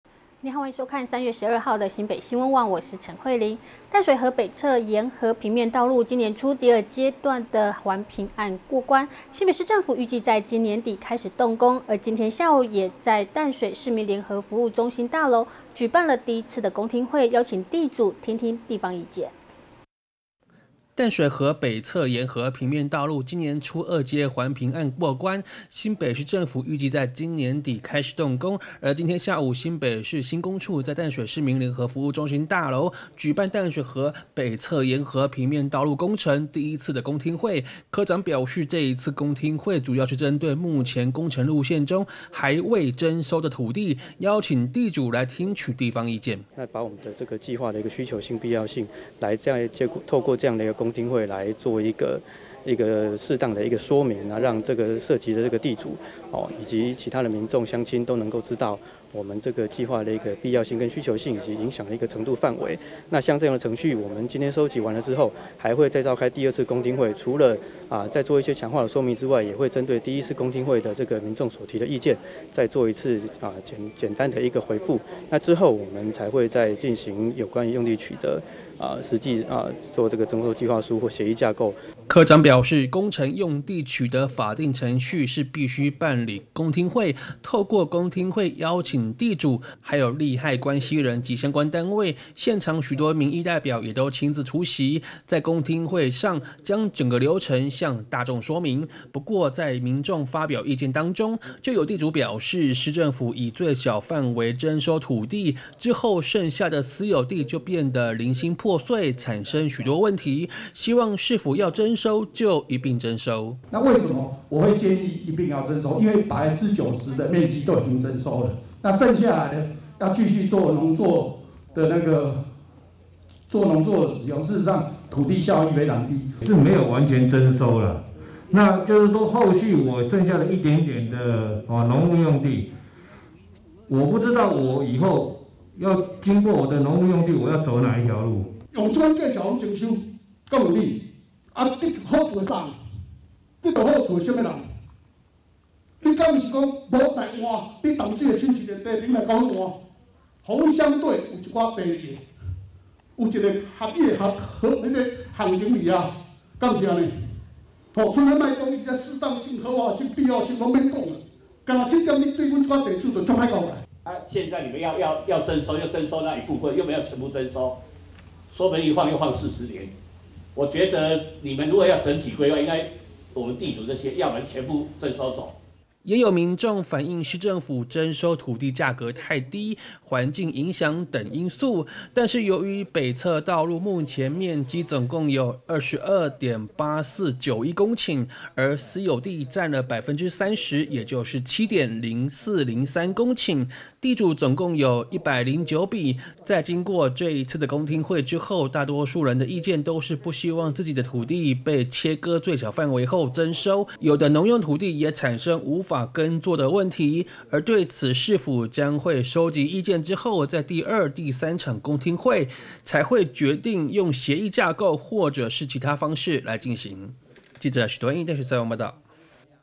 淡北道公聽會 地主不滿最小範圍徵收
淡水河北側沿河平面道路，今年初二階環評案過關，新北市政府預計在今年底開始動工，而今天下午新北市新工處在淡水市民聯合服務中心大樓，舉辦『淡水河北側沿河平面道路工程』第一次公聽會，科長表示，這一次公聽會主要是針對目前工程路線中，還未徵收的土地，邀請地主來聽取地方意見。

新北市新工處科長 李方谷表示，工程用地取得，法定程序是必需辦理公聽會，透過公聽會邀請地主，相關利害關系人以及單位，現場許多民意代表也都親自出席，在公聽會上將整個流程向大眾說明，不過在民眾發表意見當中，就有地主表示，市府以最小範圍徵收土地，之後剩下私有地就變得零星破碎，產生許多問題，希望市府要徵收就一拼徵收。

也有民眾反應市府徵收土地價格太低，環境影響等因素。由於目前北側道路面積總共有22.8491公頃，而私有地佔了30%，也就是7.0403公頃，地主共有109筆，在經過這一次的公聽會，大多人的意見都是不希望自己的土地被切割最小範圍後徵收，有的農用土地也產生無法耕作問題，對此市府將會收集意見後，在第二、第三場公聽會才會決定用協議價購或其他方式來進行。